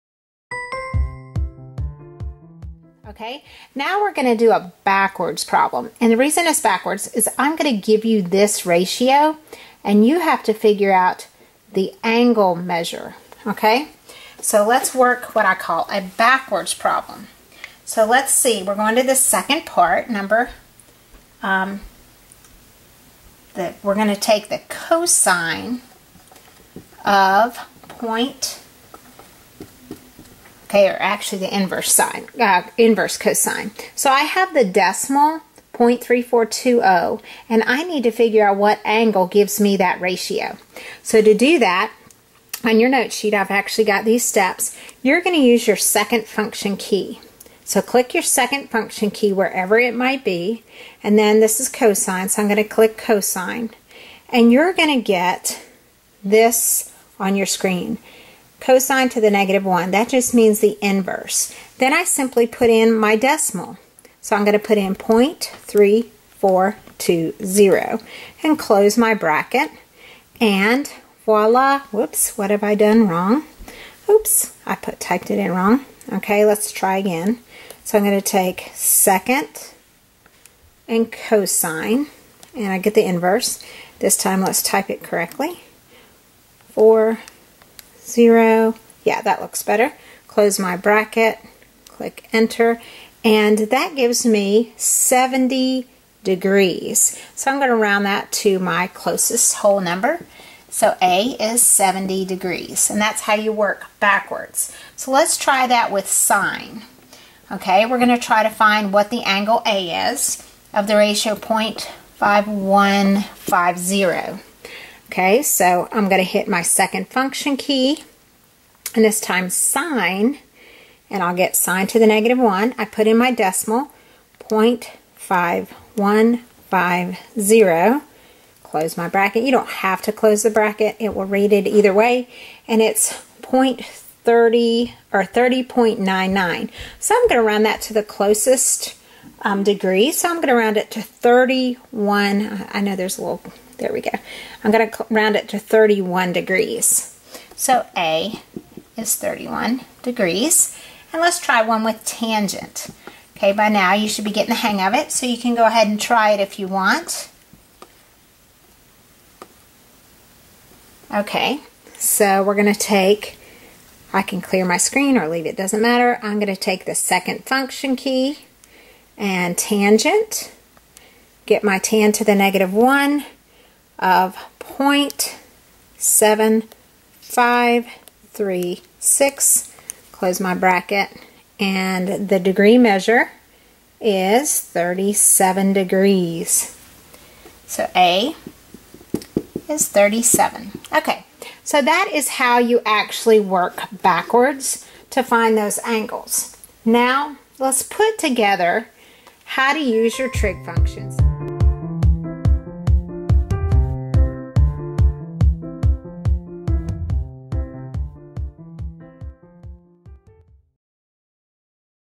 Basic Trigonometry- Angle measure from Cosine-Sine-Tangent
How to find the angle measure using a trig ratio.

Transcript
Now we are going to solve a backward problem. The reason it is a backward problem is because I will give you the ratio and you will figure out the angle measure. Let's see we will go to the second part, take the cosine of .3420 and I need to see which angle it gives me. In order to find the angle you will use your second function key wherever it might be and then cosine and you will get (cos-1 on your screen. That just means it is the inverse, and then I put in my decimal which is .342) and then close my brackets. Ok I typed it in wrong again take the second and type .3420 correctly and close brackets and I get 70.001 degrees, and I will round that to my closest whole number which is 70 degrees. That is how you work backwards.
Let's try that with sine. We are trying to find the angle A from the ratio .5150. Hit second function key, and then sine and I get sine to the negative 1, put in my decimal .5150 close brackets and I get my angle measure of 30.99. I will round this to 31 degrees which is the closest degree.
So A is 31 degrees.
Finally, we will try one with tangent. By now you should be getting the hang of it.What is the angle measure of tangent ratio .7536 Hit my second key, hit tangent, get my -1 then enter my ratio of .7536, close the bracket and I get 37 degrees. So A is 37 and that is how you work backwards to find those angles.